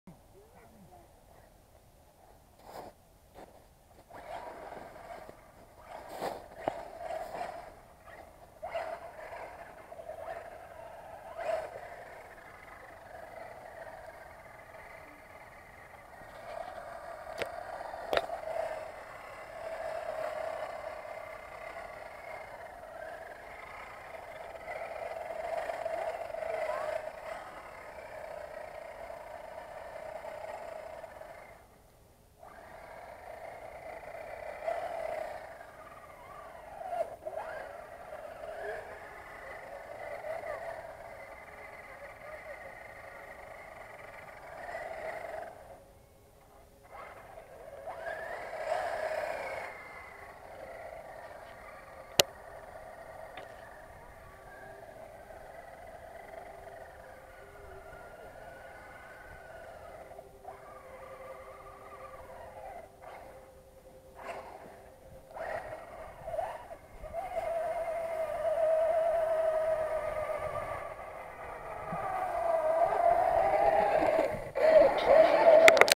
Traxxas slash 4x4 playing on thin ice.
Shot with sj4000 720p 60fps.
Driving the car on only 1cm of ice.